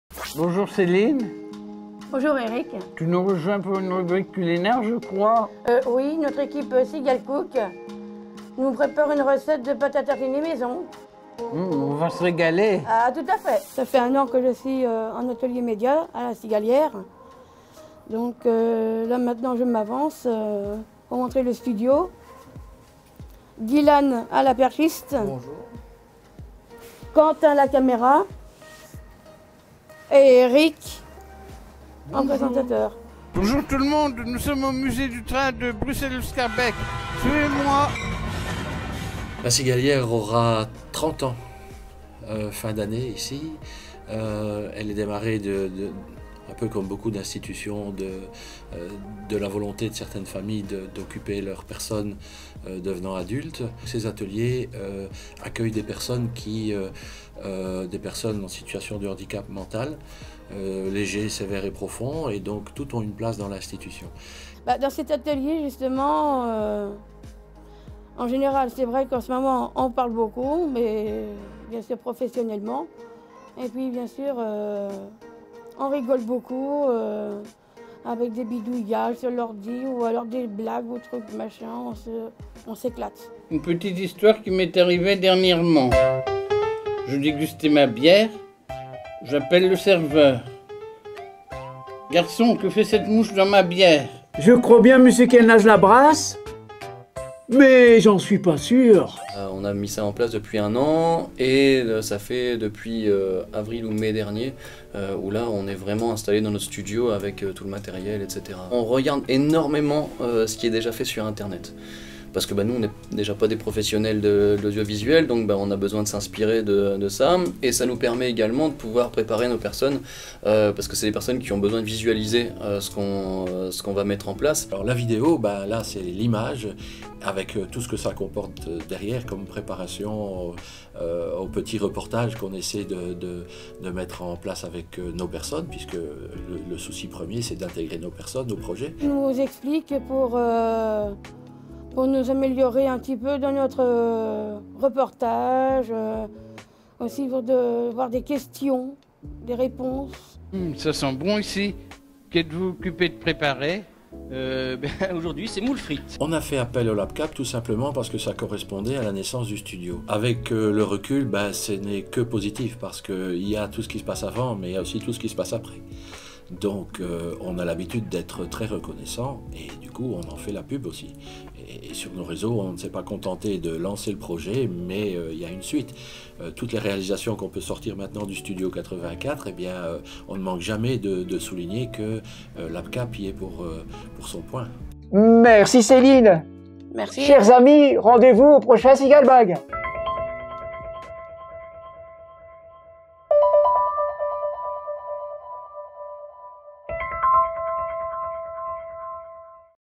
LABCAP48 : Un studio télé grâce au financement !
Vous travaillez dans une association et avez une idée géniale mais vous manquez de moyens pour la financer?
Le LabCAP48 with CBC est fait pour vous !
Comme ici, à Mouscron, où La Cigalière a pu aménager un studio télé!